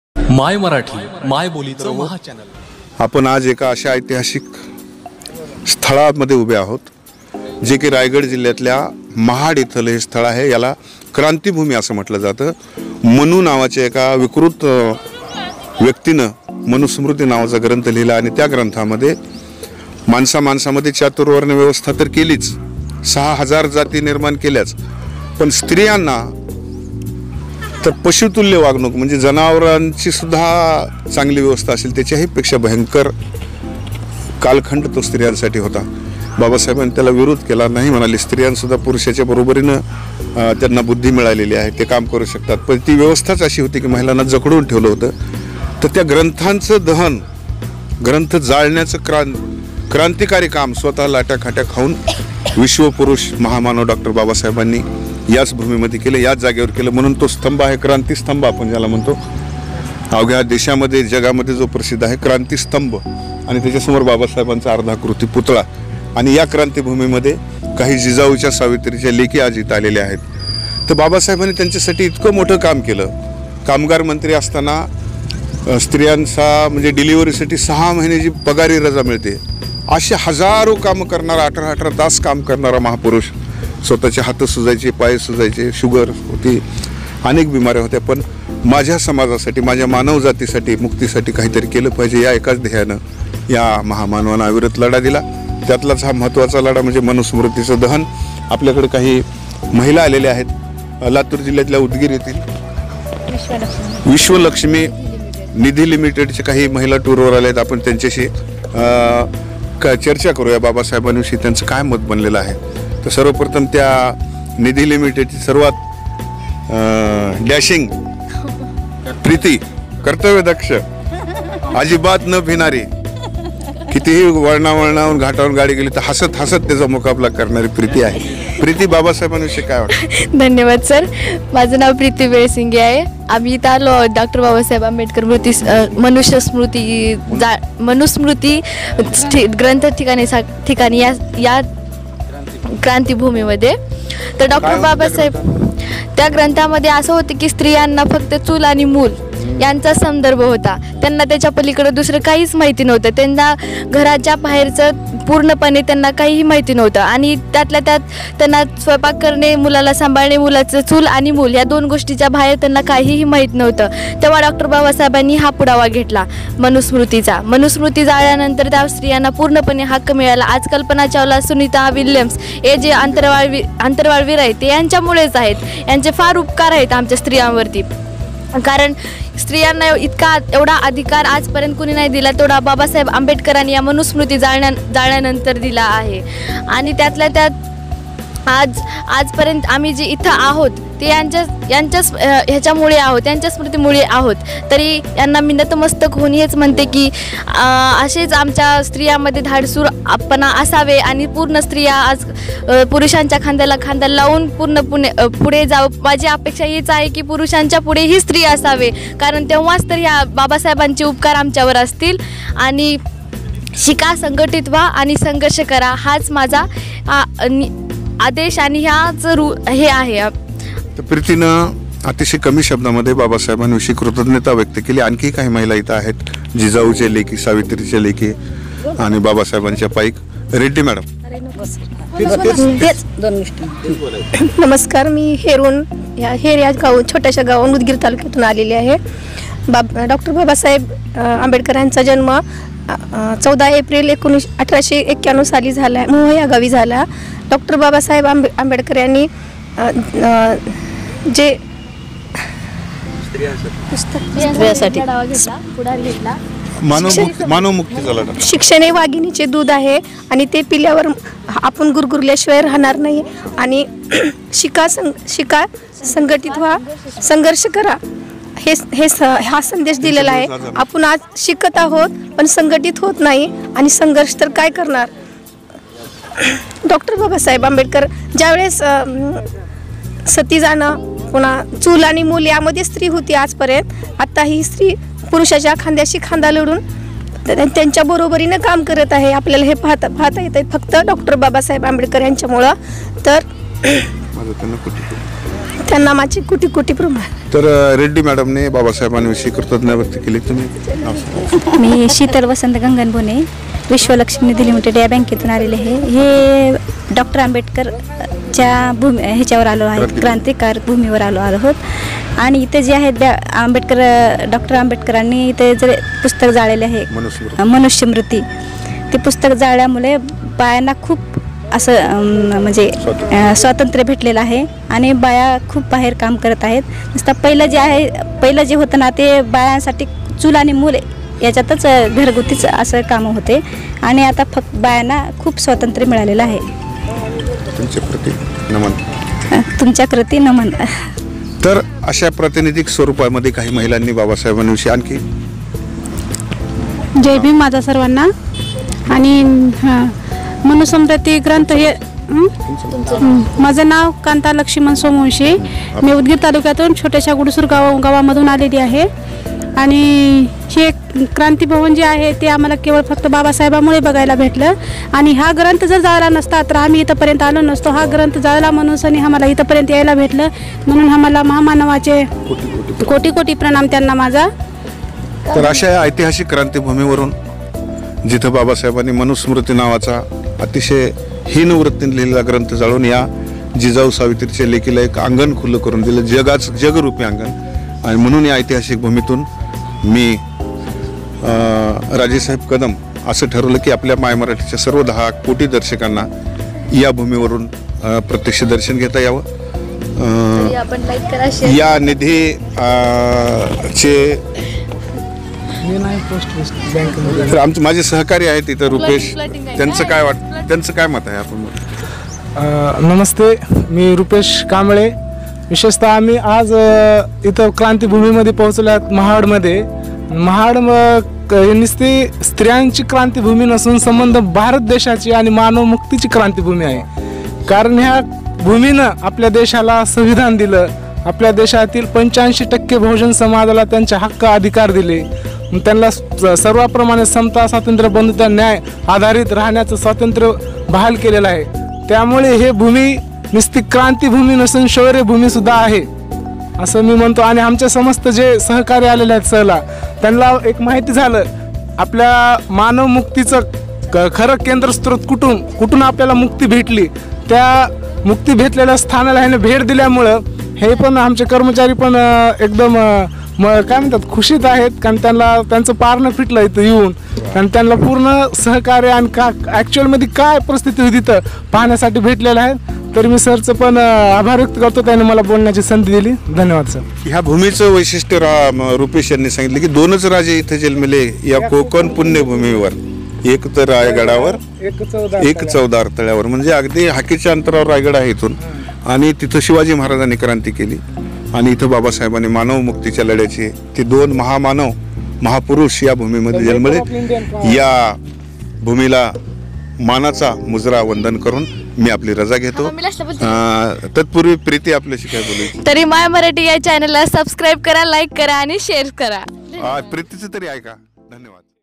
mahad,krantibhoomi,महाड,क्रांतीभूमी,dr.babasaheb ambedkar,mahad dam,live,डॉ. बाबासाहेब आंबेडकर,dj,
ज्या ग्रंथाने चातूर्वण व्यवस्था निर्माण केली,ज्या ग्रंथाने माणसामाणसांत जात नावाच्या साडेसहा हजार भिंती उभ्या केल्या.ज्या ग्रंथाने स्त्रियांना कस्पटासमान समजले त्या ग्रंथाचे दहन महामानव,विश्वरत्न डॉ. बाबासाहेब आंबेडकर यांनी महाडच्या याच मैदानात केले.
क्रांतीभूमी म्हणून ओळखल्या जाणा-या थेट या मैदानातून हा खास रिपोर्ट खास आपल्यासाठी.

'माय मराठी '.
मायबोलीचं महाचॅनल. !!

mai Marathi...
the biggest channel of the mothertoung.
-rajesaheb kadam